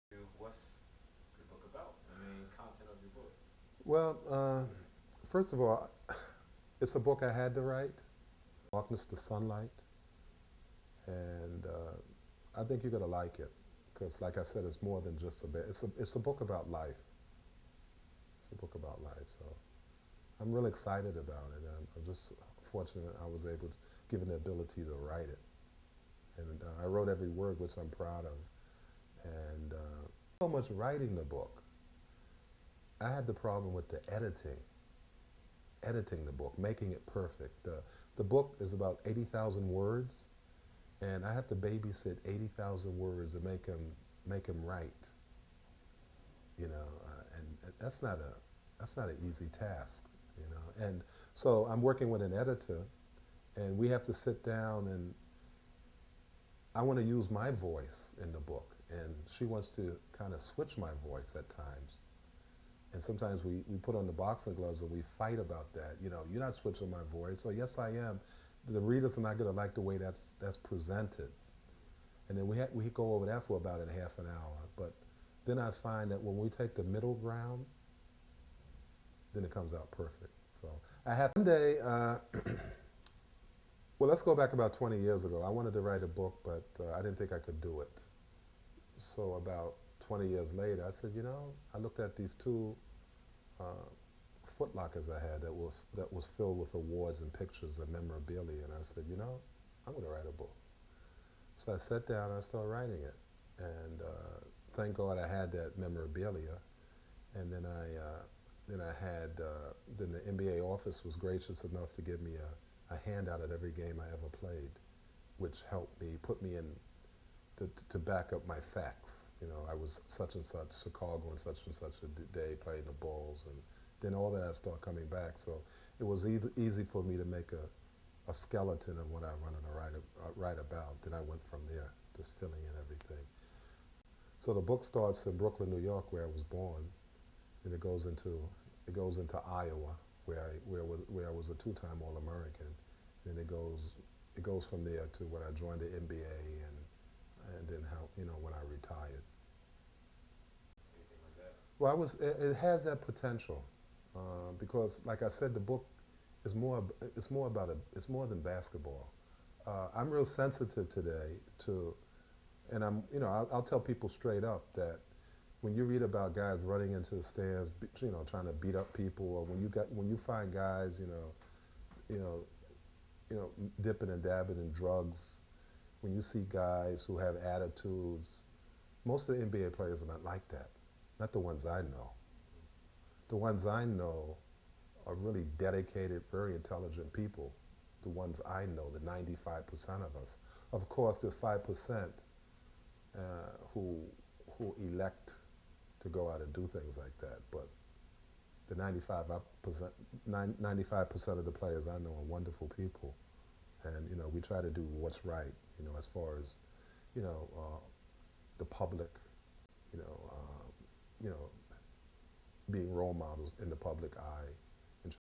Zaid Abdul-Aziz, Author of Darkness to Sunlight
Zaid Abdul-Aziz talks about why he had to write "Darkness to Sunlight", learn more about this ex-NBA player/author by visting